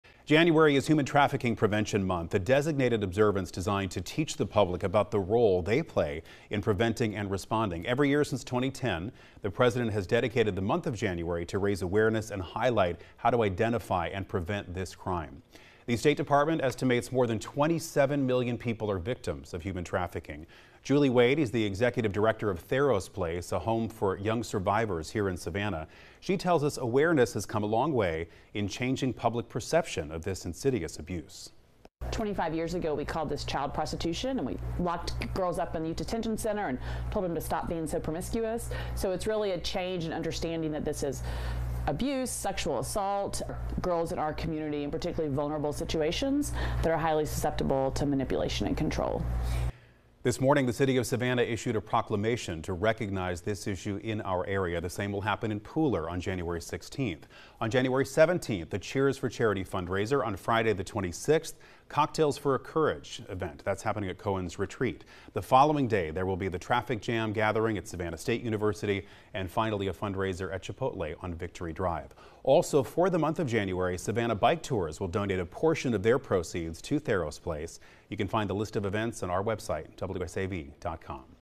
Fundraisers planned to support Tharros Place
Tharros Place provides shelter and support for trafficking survivors.